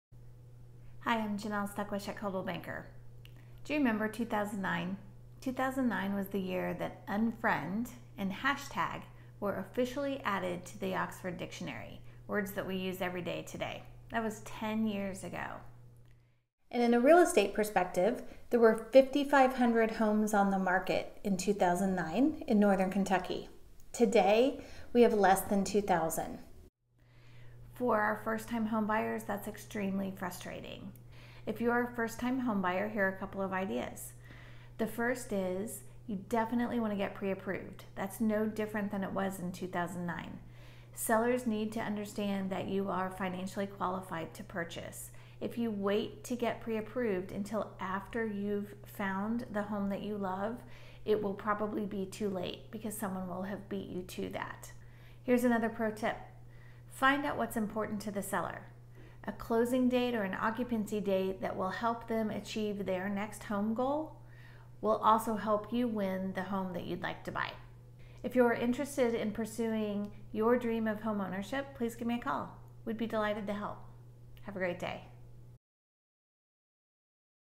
ust Ask Janell: Episode 5 - "How Do I Win My Dream Home with Low Inventory?"
🏘 Buyers, especially first-time home buyers, want to know how to win their dream home when the inventory is low!

👇 Well, my friends, I have a few tips for you in this week's "Just Ask Janell".